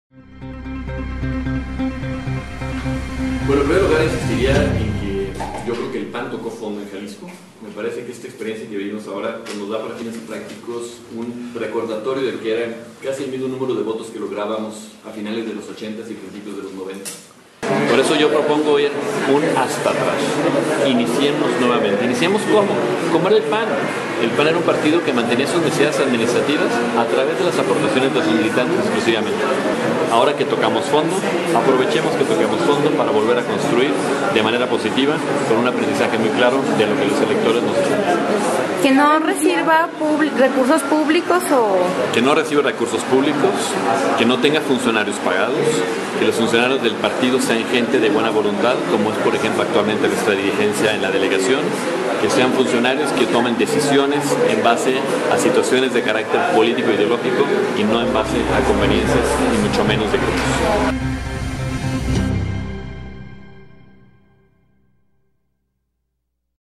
El PAN tocó fondo: Petersen
Que renuncie a sus prerrogativas el partido, sugiere quien fue candidato a Guadalajara en estas elecciones